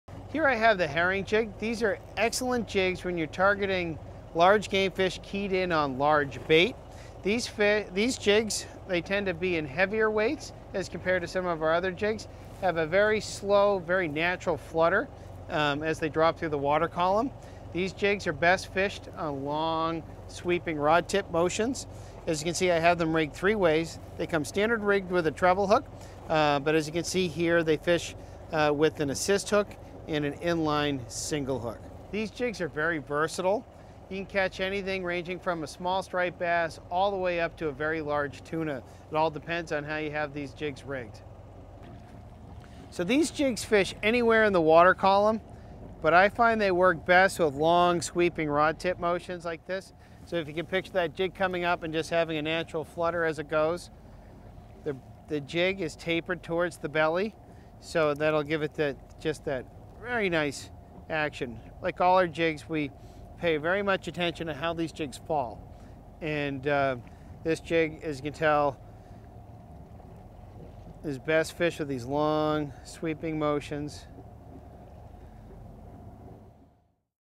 How To: Hogy Herring Jig
This is our go-to metal when fish are keyed in on larger profile forage. Available in 3.5oz and 4.5oz weights, the Herring Jigs are a perfect upgrade from traditional Diamond Jigs. Sporting lifelike holographic baitfish finishes and a unique herring body shape. These versatile jigs have an excellent side to side kick on a surface retrieve, yet double down with controlled flutter on a vertical presentation.

Captain: Capt. Mike Hogan
Boat: Fortier 30
Technique: Jigging (long, sweeping rod tip motions)
Targeted Species: Large game fish keyed in on large bait
Lure Used: Hogy Herring Jig